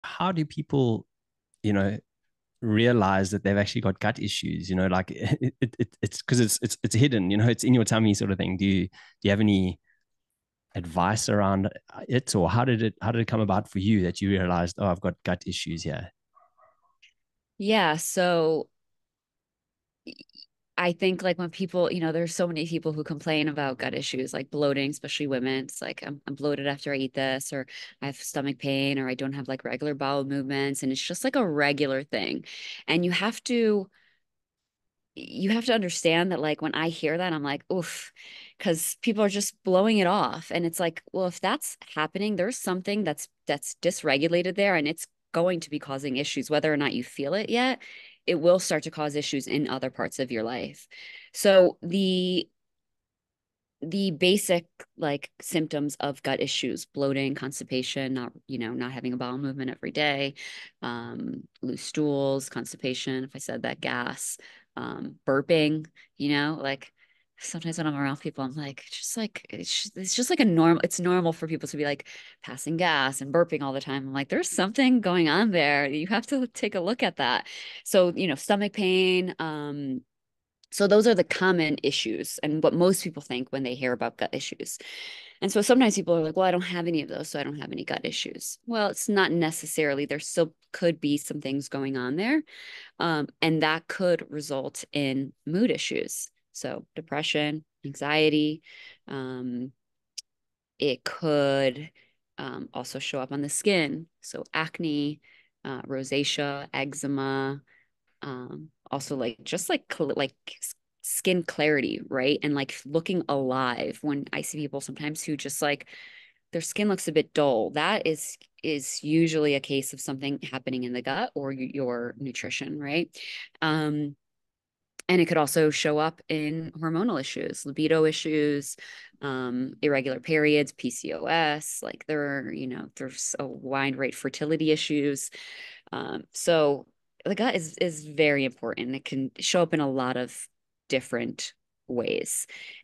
Resolving Your Bloating, Constipation and other Gut Issues
How do you, as a woman, know when you have gut issues?

Basic symptoms of gut issues are:

bloating
constipation
not having a bowel movement every day
loose stools
constipation,
gas
burping

AMANDA CARNEIRO

RIDICULOUSLY HUMAN SOCIAL MEDIA ACCOUNTS